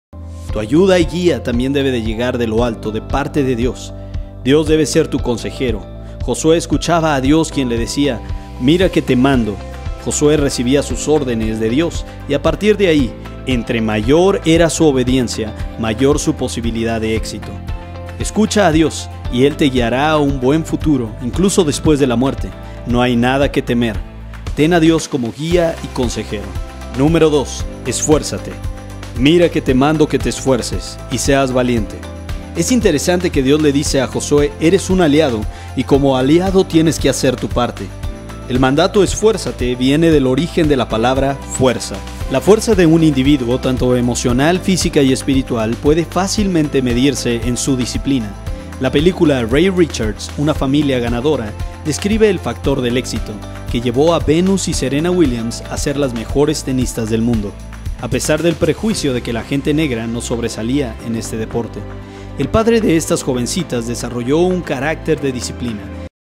🏹La Estrategia de Emprendimiento al Éxito⚔ Switch Reflexión🛡 4
Tenemos los nervios de punta que nos mantienen al tanto de los noticieros que parecen ser cultivos de malas noticias. ¿Qué hacer ante un futuro que no parece muy prometedor? Esa es la pregunta que hoy contestaremos quédate con nosotros y al final del video se que tendrás una Clara estrategia y los pasos a tomar para comenzar un emprendimiento al Éxito .  La Estrategia de Emprendimiento al Éxito